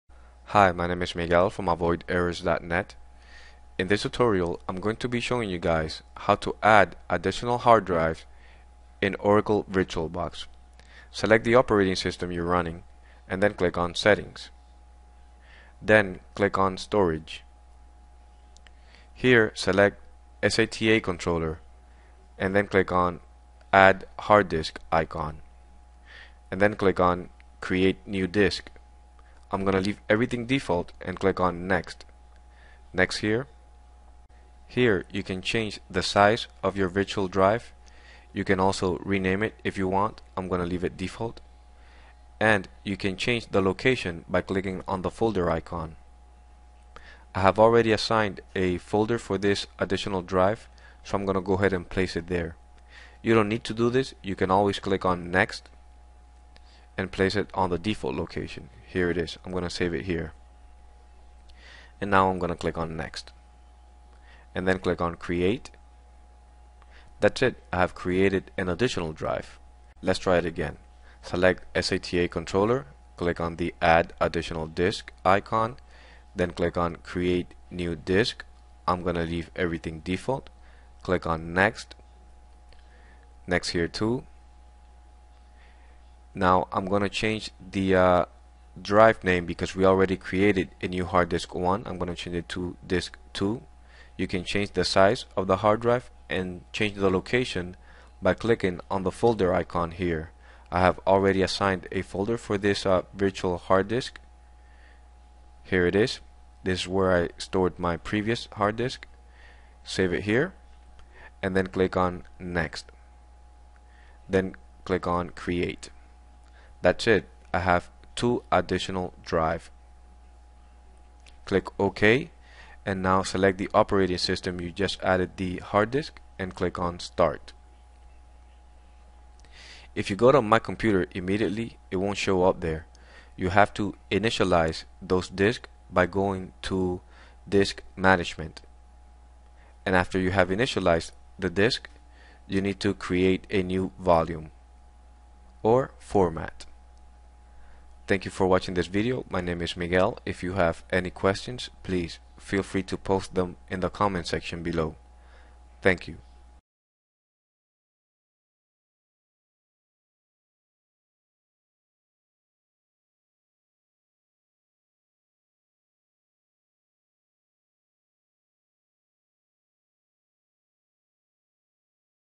How to add Hard Disks in Oracle VirtualBox
We will learn how to add hard disks into an existing virtual machines. This is identical to adding hard disks to a physical machine and can be used for a variety of reasons: more data storage, better performance or increased reliability via RAID setup and others.